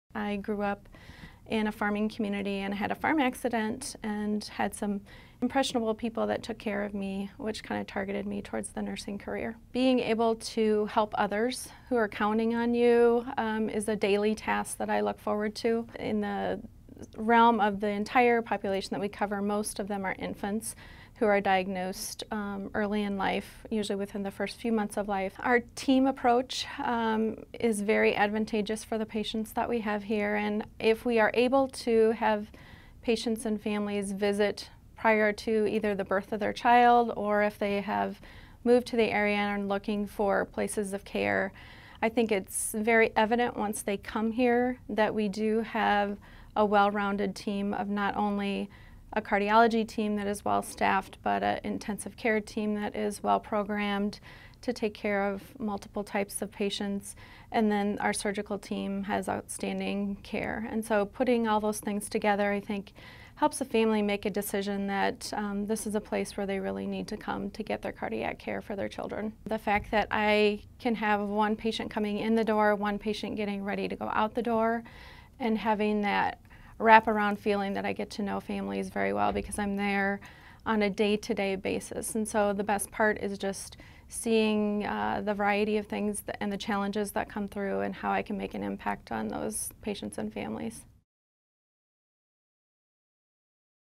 Trudy Pierick, ARNP
Trudy is a pediatric nurse practitioner at University of Iowa Stead Family Children's Hospital.